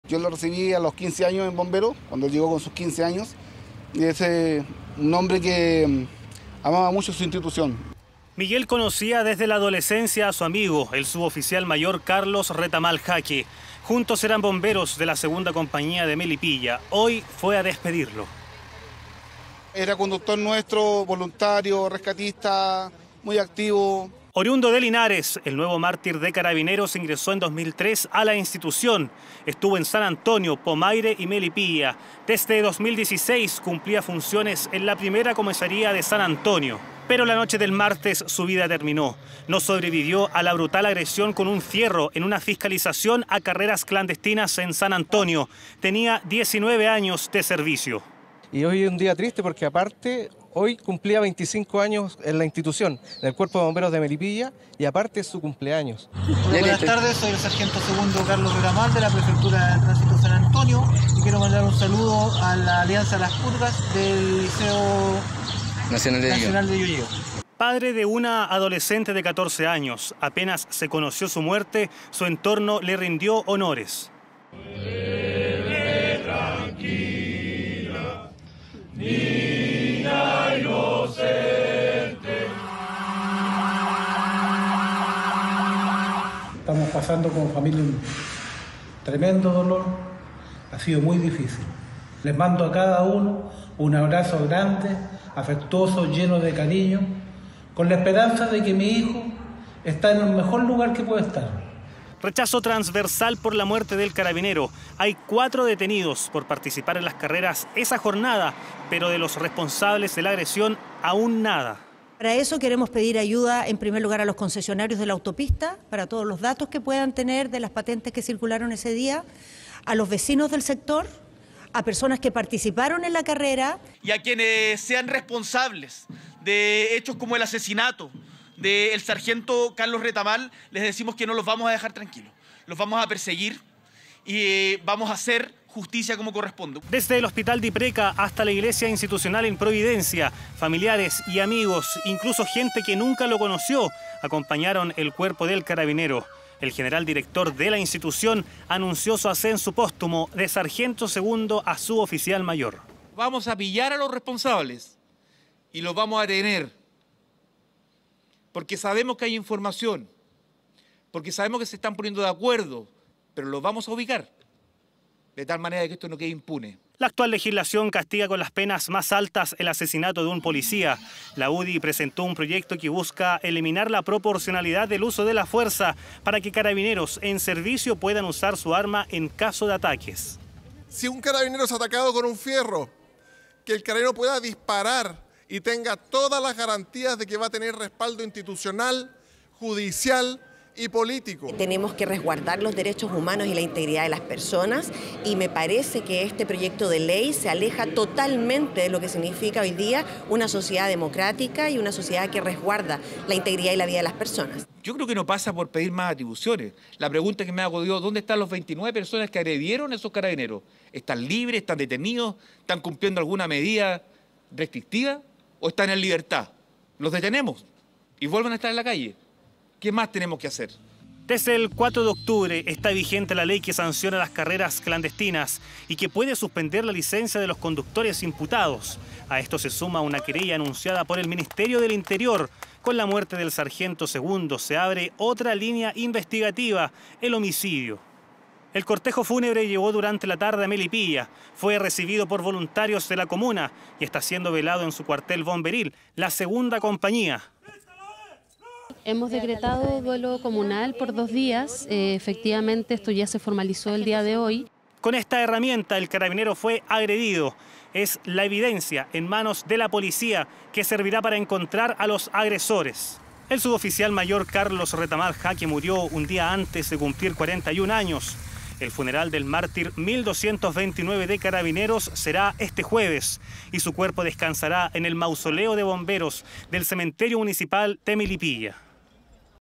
La singular historia de vocación del carabinero bombero: no pudo resistir a la brutal agresión
En su cuartel de bomberos de Melipilla es velado el suboficial mayor Carlos Retamal Jaque, quien murió luego de ser agredido en un control a carreras clandestinas en San Antonio. El ministerio del Interior anunció una querella contra quienes resulten responsables, aunque hasta el momento la investigación sigue sin detenidos por el crimen.